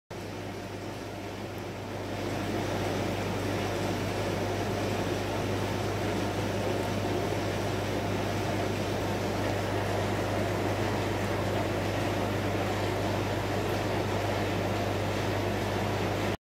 WET-AQUA Paratilapia polleni
Paratilapia polleni - a pair that looks after the roe in a large pot.